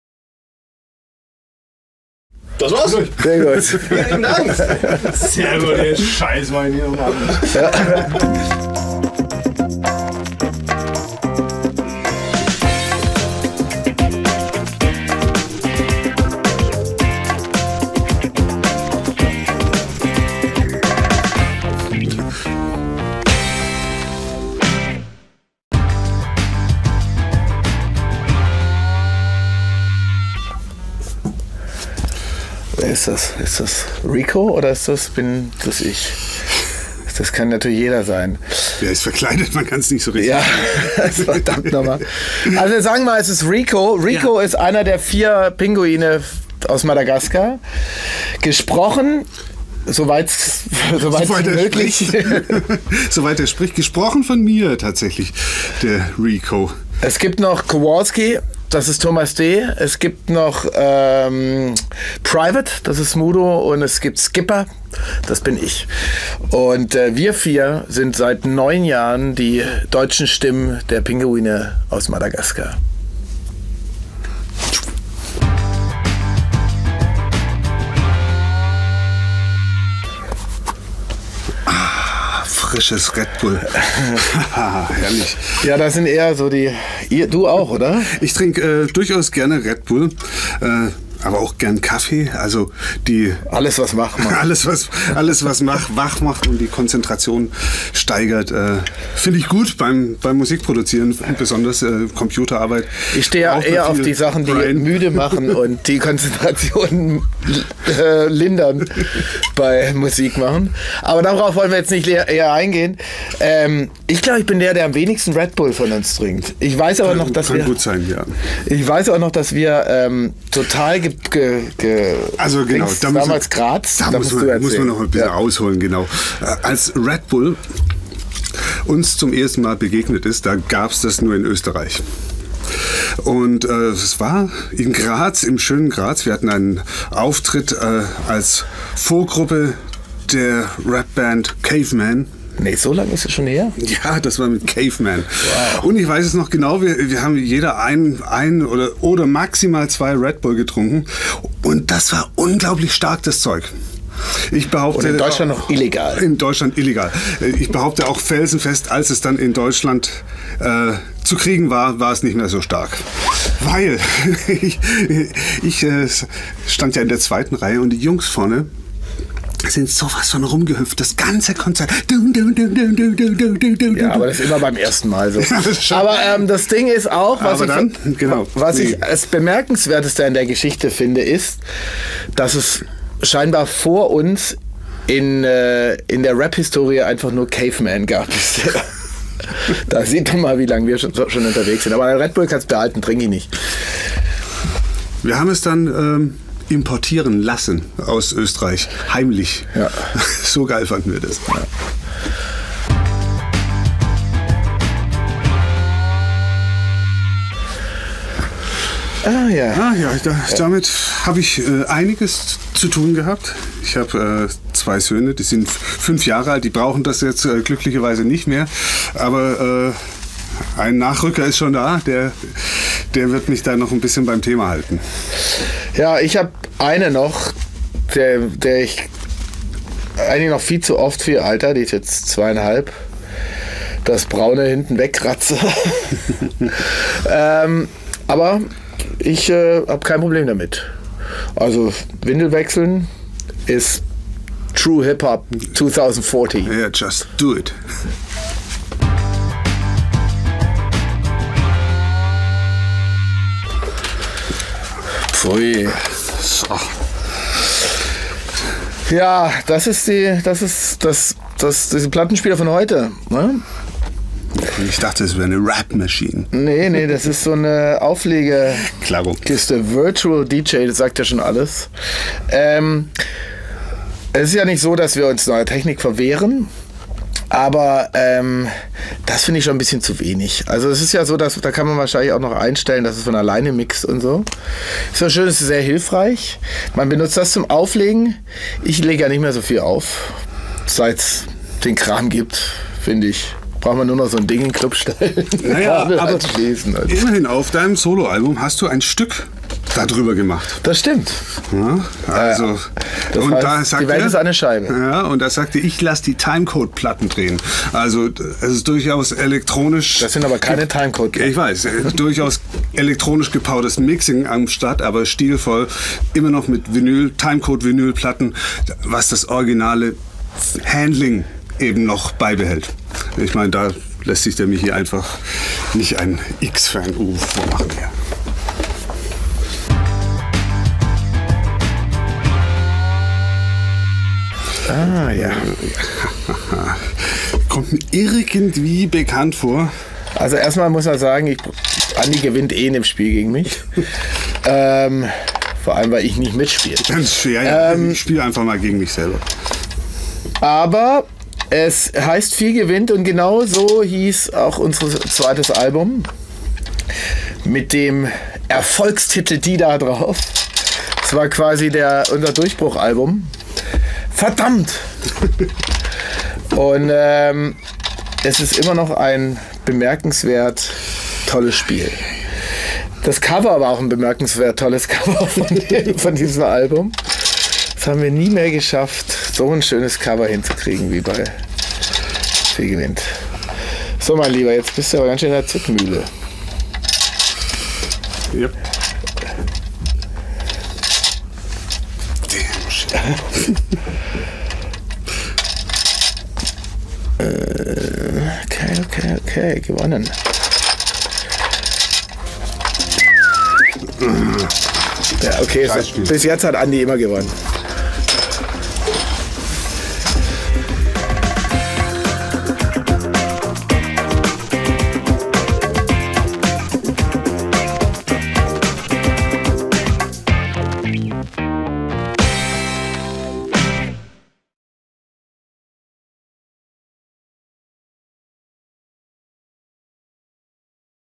Radio Emscher Lippe-Fünf für...And.Ypsilon und Michi Beck von "Die Fantastischen Vier"
Zwei von Vieren hatten wir schon, jetzt sind die anderen beiden dran: Fünf für And.Ypsilon und Michi Beck von "Die Fantastischen Vier"!